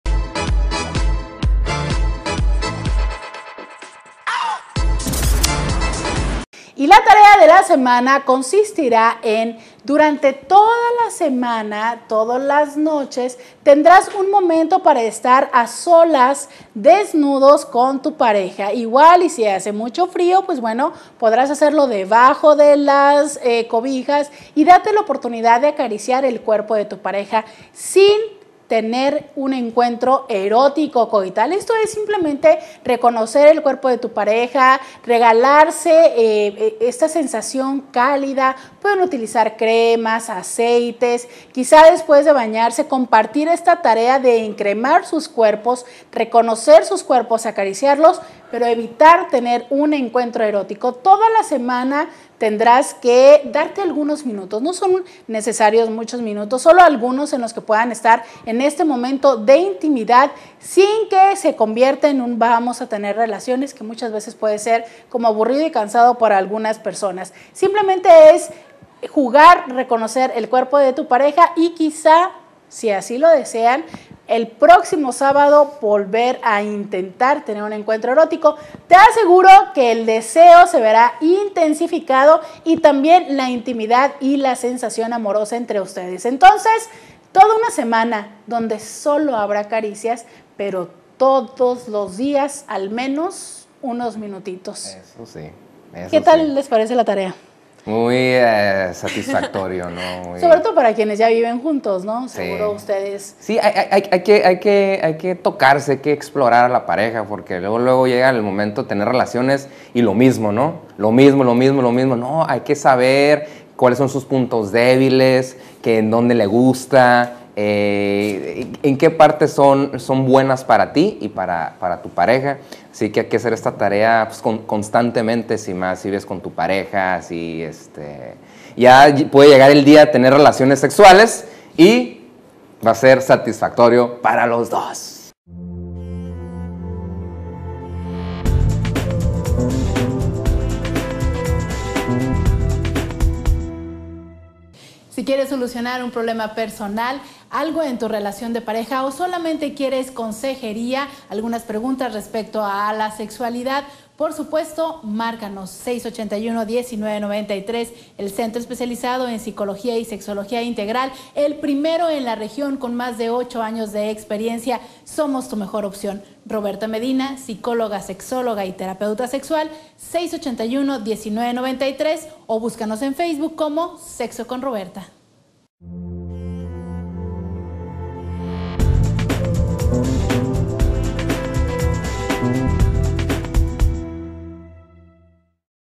Sexo con Robertha: Tarea Sexual de la Semana
Robertha Medina Psicóloga y Sexóloga hablando de: la tarea sexual de la semana.